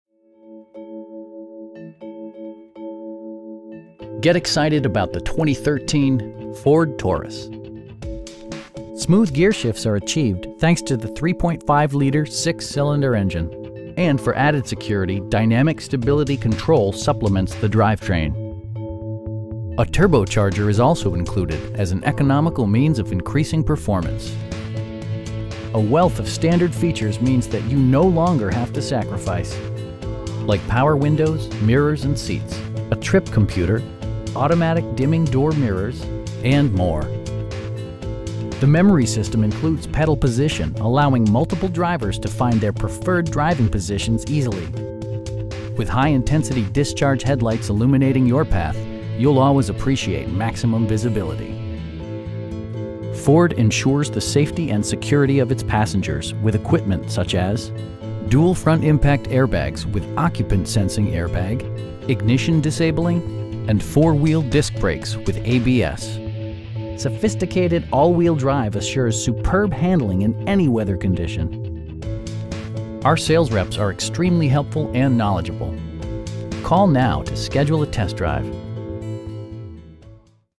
2013 Ford Taurus SHO in Cincinnati, OH 45238
Walt Sweeney Glenway Ave
5400 Glenway Avenue

Get excited about the 2013 Ford Taurus. This 4 door, 5 passenger sedan provides exceptional value! Smooth gearshifts are achieved thanks to the 3.5 liter 6 cylinder engine, and for added security, dynamic Stability Control supplements the drivetrain. A turbocharger is also included as an economical means of increasing performance. A wealth of standard features means that you no longer have to sacrifice. Like power windows, mirrors, and seats, variably intermittent wipers, a trip computer, an automatic dimming rear-view mirror, automatic dimming door mirrors, front bucket seats, an overhead console, and more. Features such as automatic climate control and leather upholstery prove that economical transportation does not need to be sparsely equipped. The memory system includes pedal position, allowing multiple drivers to find their preferred driving positions easily. With high intensity discharge headlights illuminating your path, you'll always appreciate maximum visibility. Ford ensures the safety and security of its passengers with equipment such as: dual front impact airbags with occupant sensing airbag, front side impact airbags, traction control, brake assist, ignition disabling, and 4 wheel disc brakes with ABS. Sophisticated all wheel drive assures superb handling in any weather condition. Our sales reps are extremely helpful & knowledgeable. We'd be happy to answer any questions that you may have.